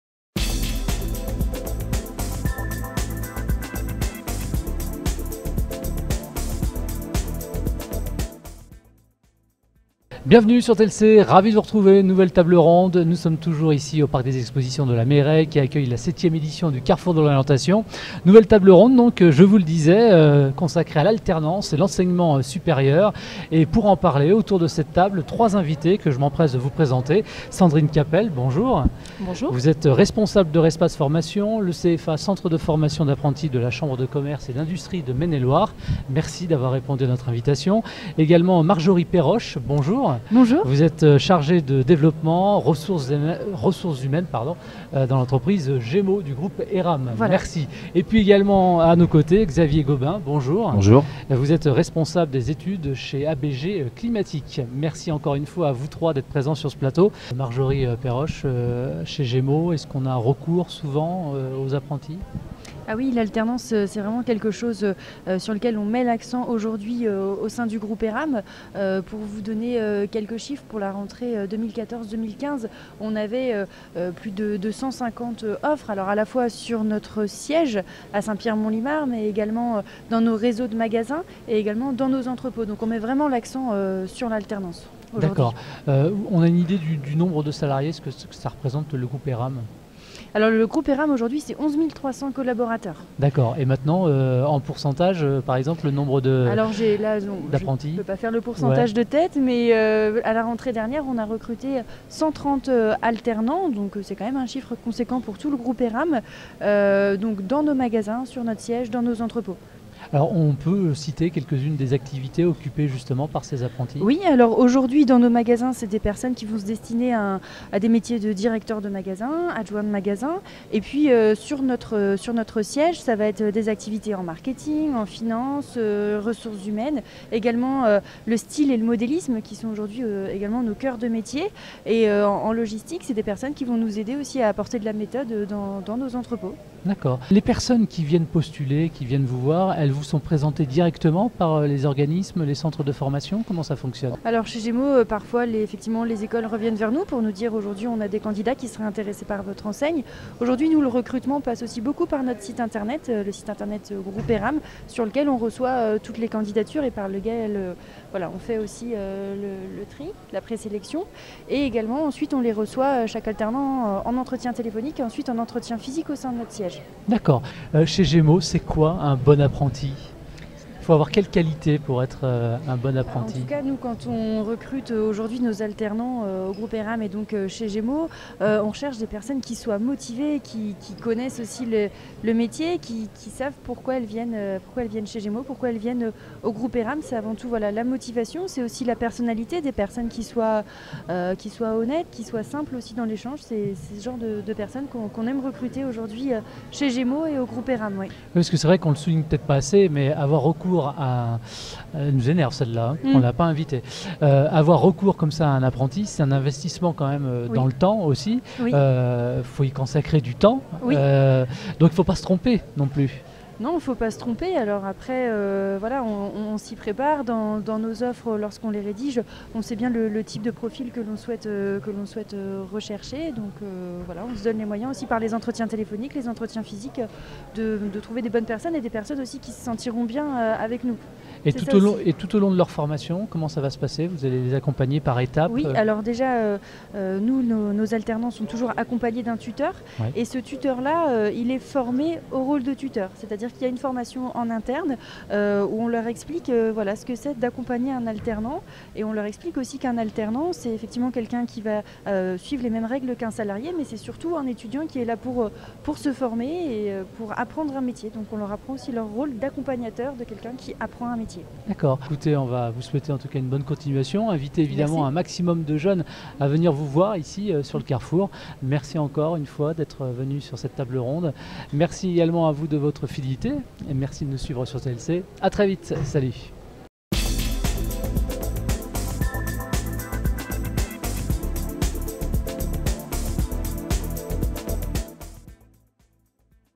L'alternance chez GÉMO
Dans le cadre du carrefour de l’orientation organisé à Cholet les 15, 16 et 17 janvier 2015, nous avons participé à une table ronde sur le thème de l’alternance dans l’enseignement supérieur.
L’occasion de rappeler la politique menée par Gémo dans ce domaine, et la volonté de l’enseigne de découvrir et former de nouveaux talents !